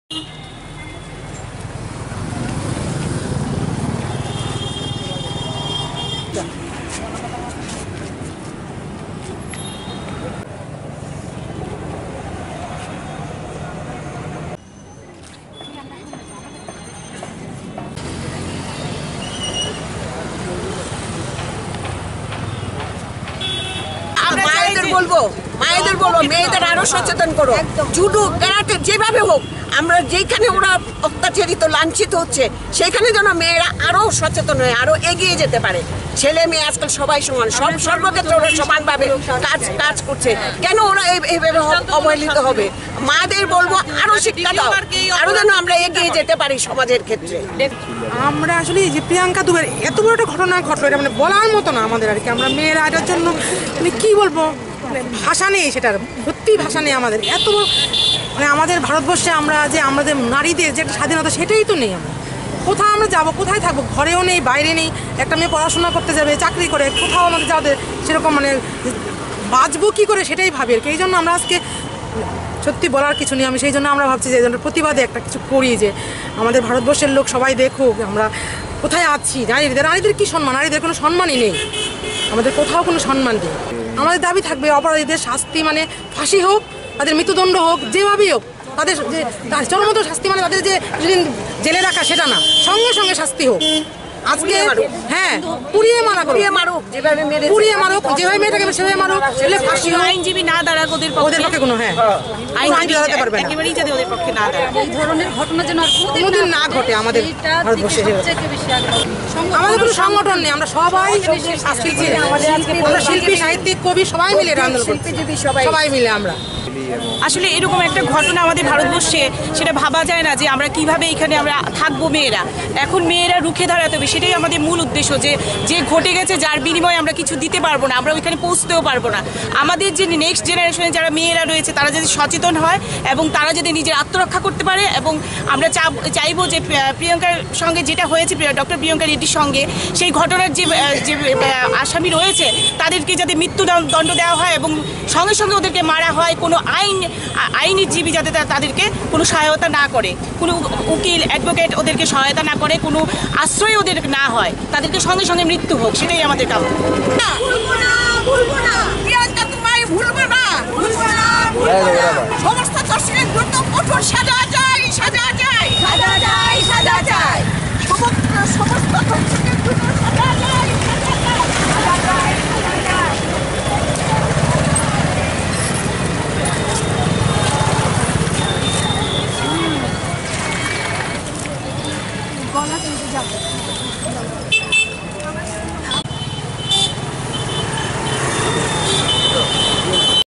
Civil society members condemn rape of Hyderabad based vet in the city
Civil society members condemn rape of Hyderabad based vet in the city on December 02